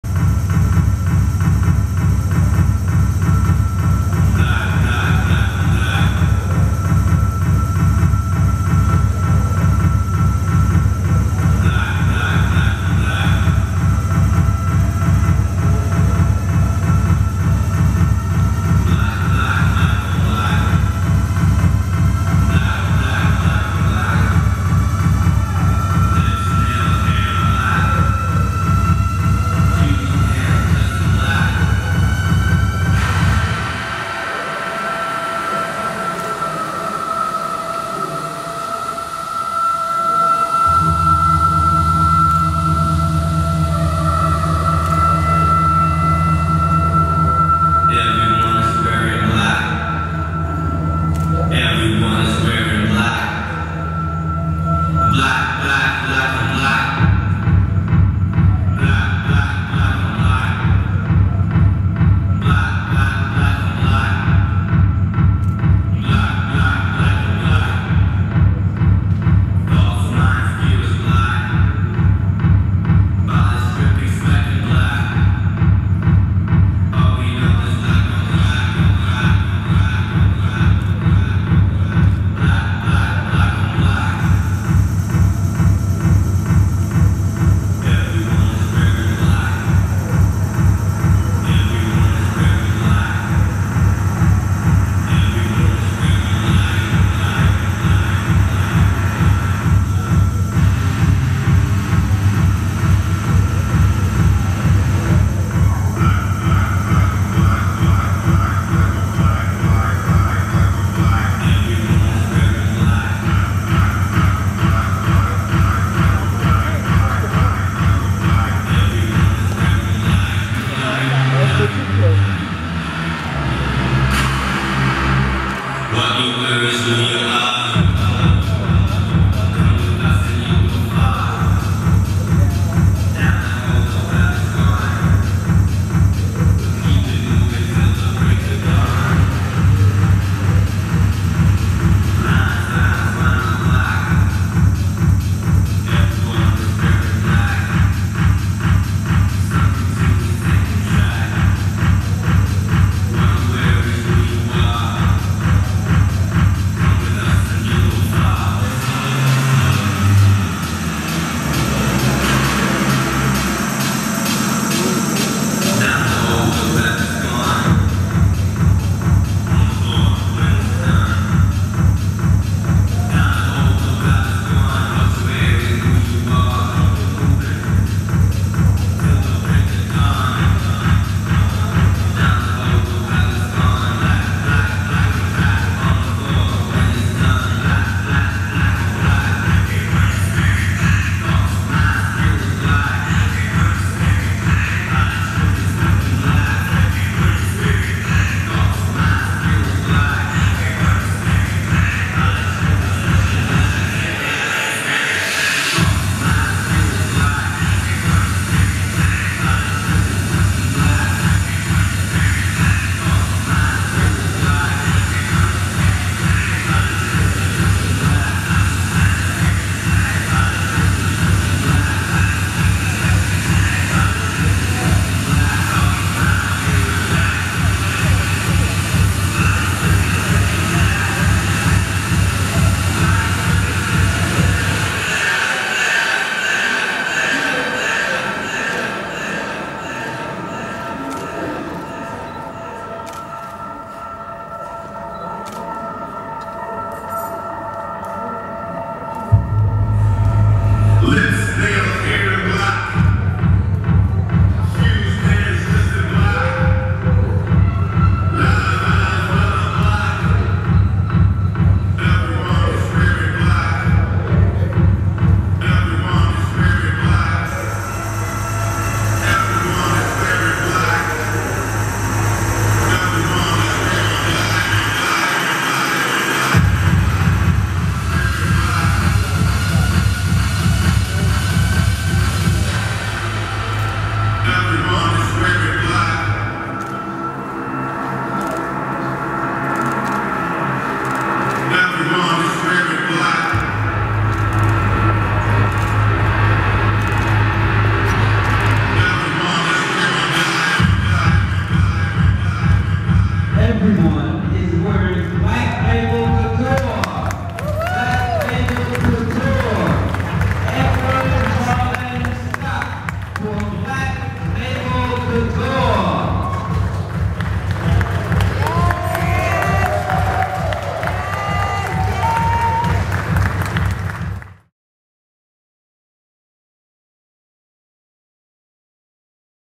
Black Label Couture VAFW 2023
Designer Black Label Couture by Edward Harlan Scott @ VA Fashion Week 2023

Follow on Instagram @vafashionweek